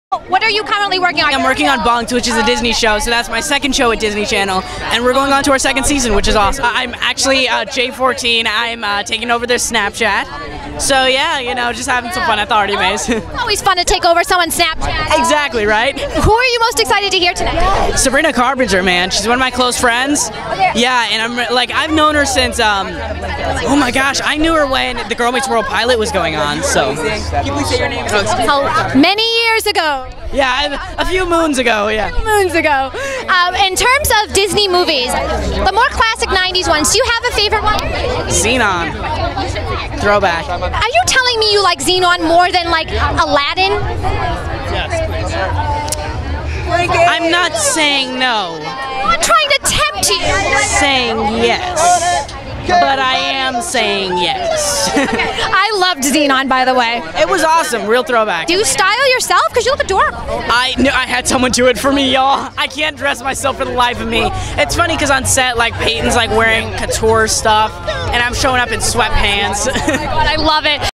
Karan Brar @ The 2016 Radio Disney Music Awards
Ashley Daniels interviews Karan Brar at the 2016 Radio Disney Music Aw